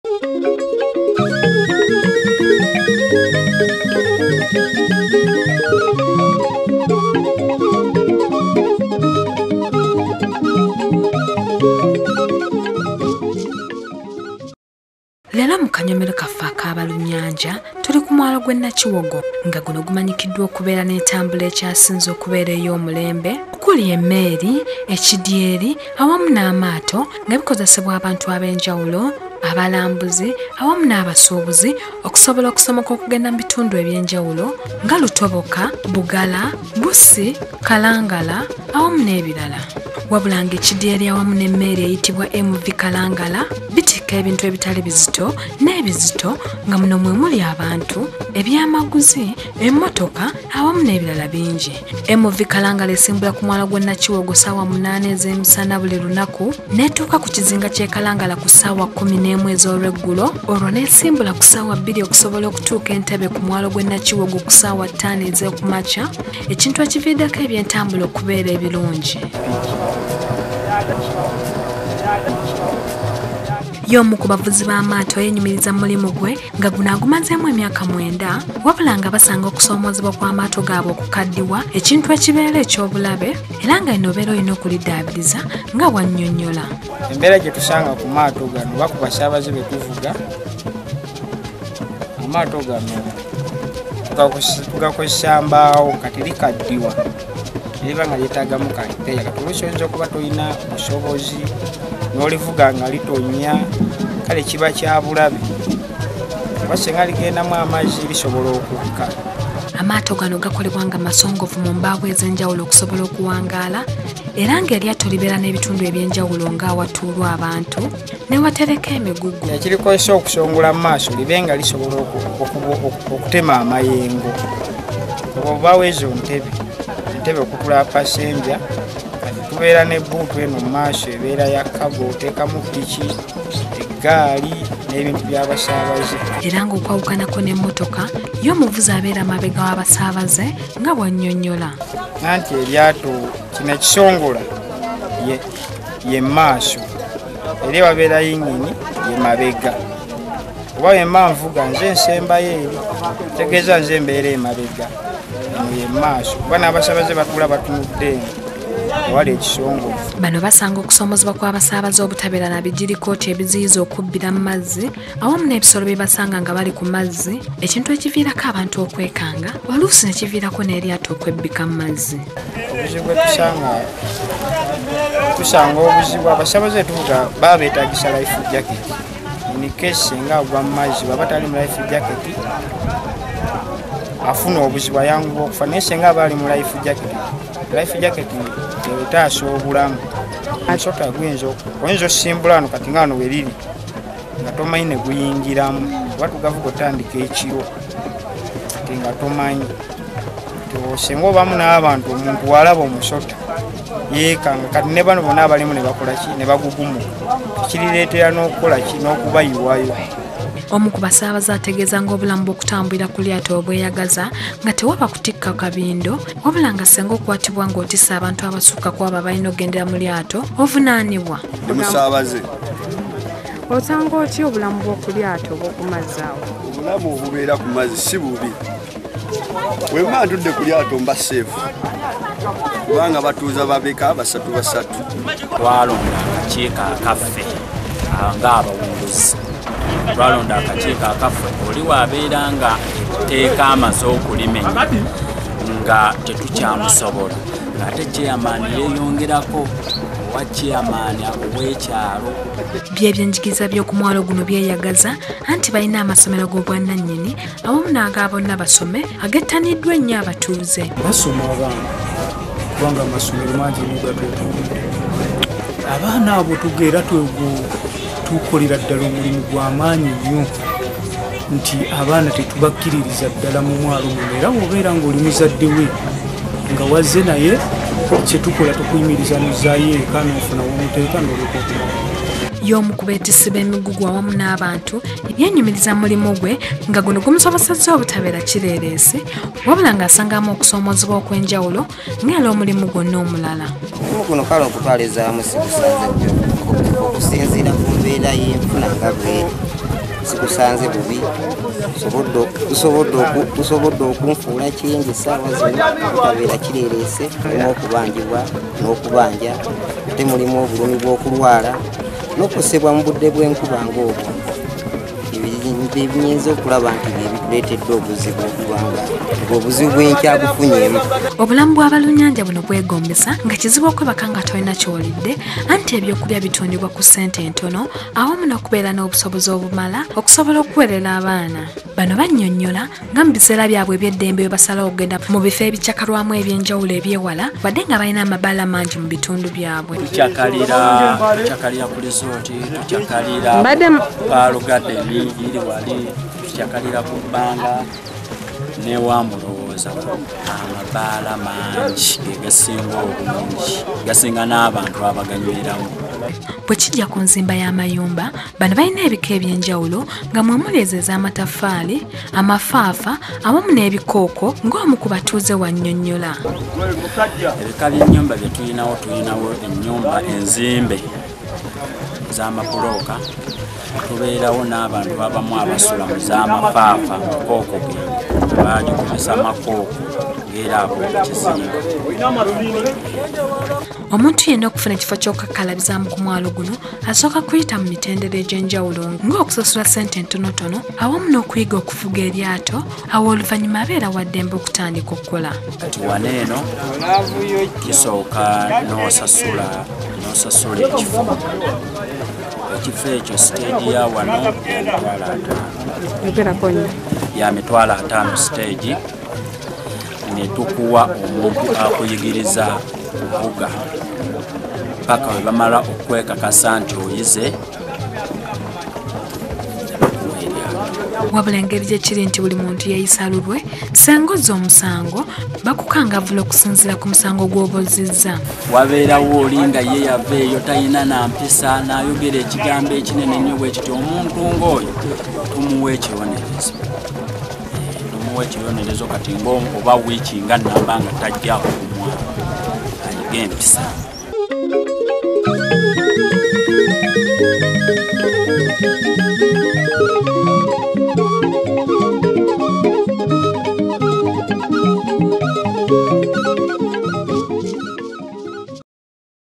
Abalunnyanja: Eby'entambula e Kigungu ku mwalo byamulembe
N'okukwasisa amateeka n'empisa nabyo binyuwevu.